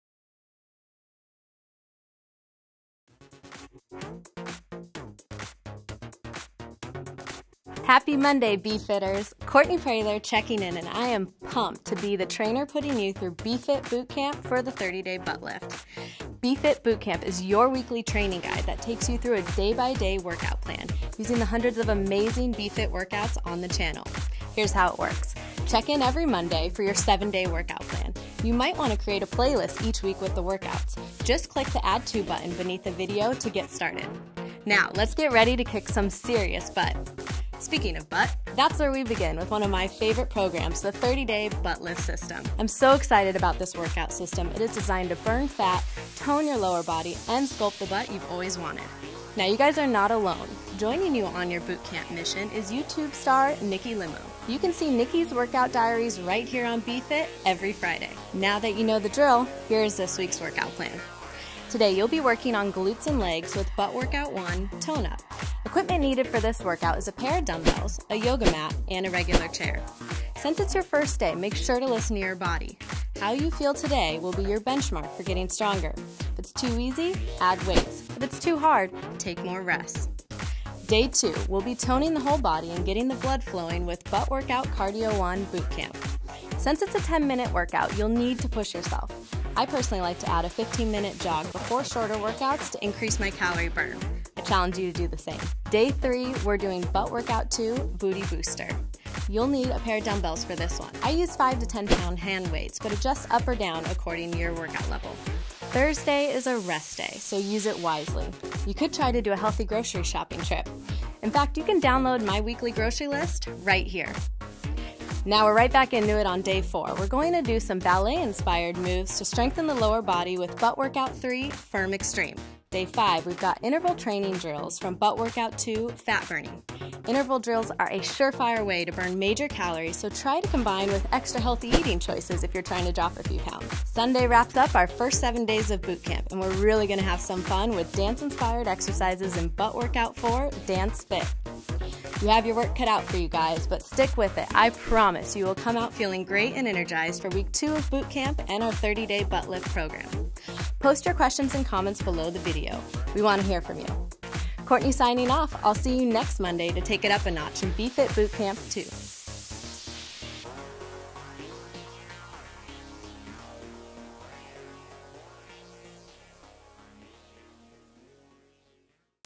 30 Day Butt lift Workout Plan: Week 1 - BeFit Bootcamp
30 Day Butt lift Workout Plan: Week 1 from BeFit Bootcamp is the first installment of your weekly training guide that takes you through a day by day workout plan using the effective, booty-shaping workouts of popular "30 Day Butt Lift" Series from BeFiT. Get ready to slim the waistline, tone the abs, and shape the legs and buns with Expert Trainer & Fitness Model, Courtney Prather as she guides you through the first week of this unique, body-sculpting BeFiT Bootcamp mission. Check in every Monday for your 7 day workout plan. Consider creating a playlist each week to include the featured workouts. Burn fat, tone your lower half, and sculpt the strong and sexy body you have always wanted. This plan will firm the glutes and legs, increase circulation, strengthen abs and core, and kick start your weight loss potential through weight training, cardio exercise and interval training. Joining you on your Bootcamp mission is YouTube Star, Nikki Limo who will be uploading workout diaries every Friday that will detail her experience as she goes through the workout program. Be sure to listen to your body as you go into day 1. How you feel on the first day will be your benchmark for getting stronger. Modify to adjust difficulty level by adding or removing weight and increasing or decreasing rest periods. Consider adding a 15 minute jog before your workout to up your calorie-burning potential. Look and feel your best as you train with a pro on your own time, right from your living room. See below for links to this week's featured workouts as well as your daily plan breakdown. Click here for all of the 30 Day Butt Lift Workouts and Good luck!
Week 1 Workout Plan:
Day 4-  Rest Day- consider making a healthy grocery run

For the full 30 Day Butt Lift Workout Plan,